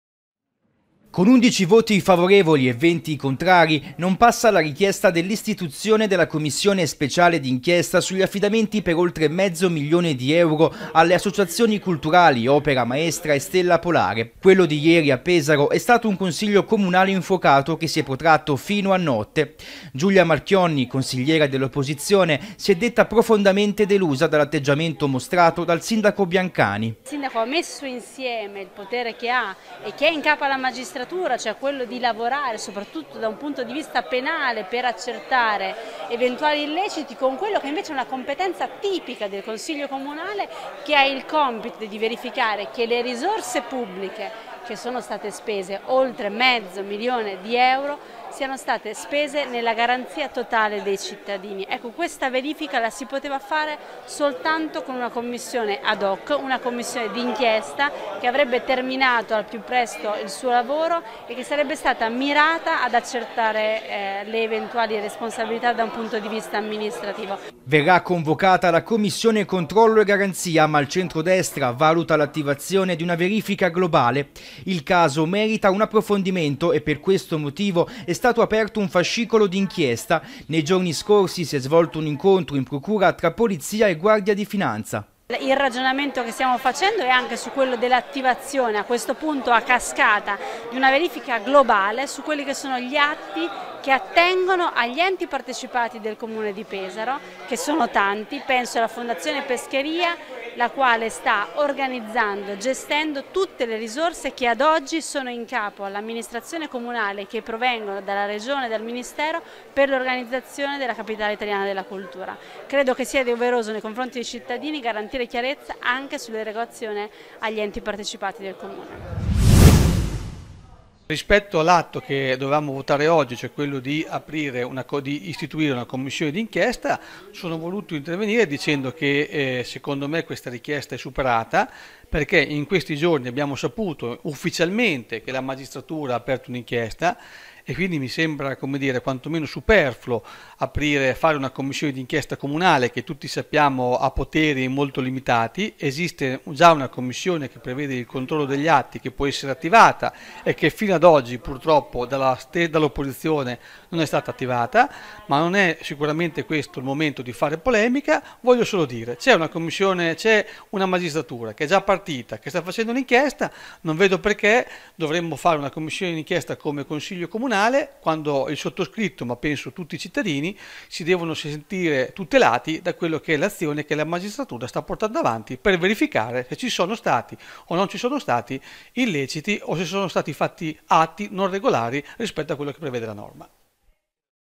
Pesaro, fondi associazioni: bocciata commissione d’inchiesta
Oltre mezzo milione a due associazioni culturali di Pesaro. Ieri in consiglio comunale la maggioranza ha votato contro la richiesta dell'istituzione di una commissione d’inchiesta